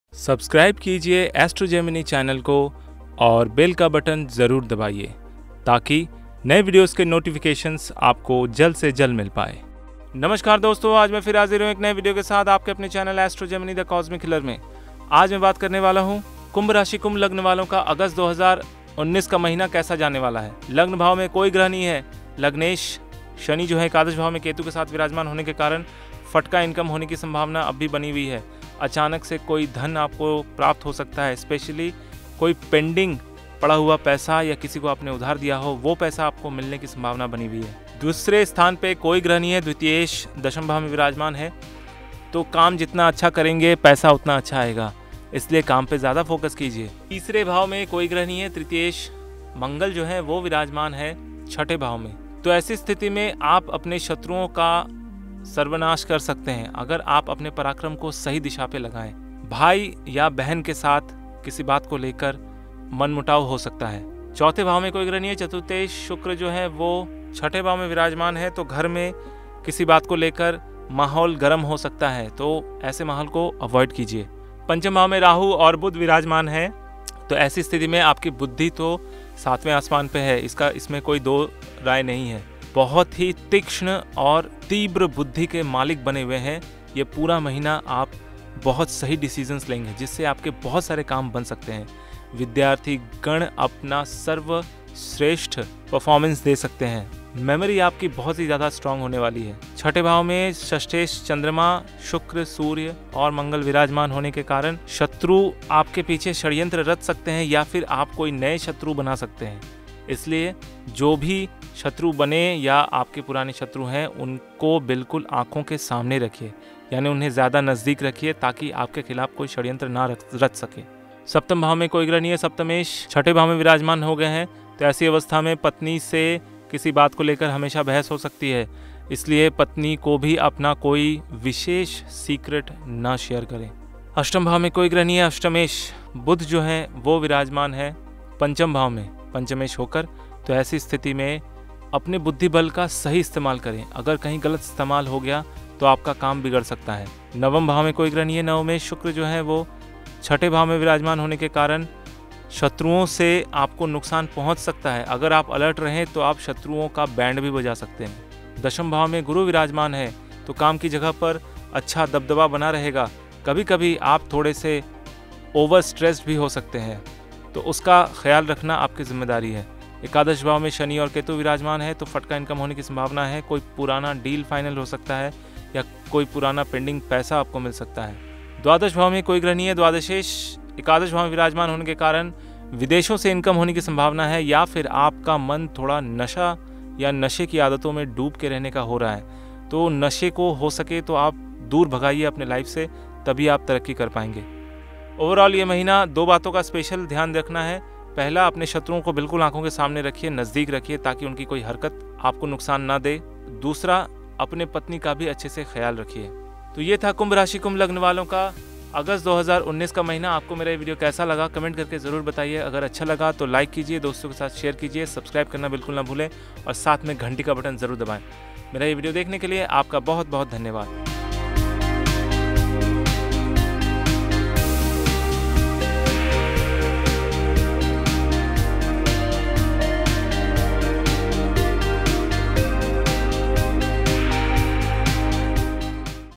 कुम्भ राशिवालों का अगस्त 2019 का महीना कैसा जानेवाला है ? ज़रूर देखिये |Aquarius Horoscope august
all kind of astrology and vastu information.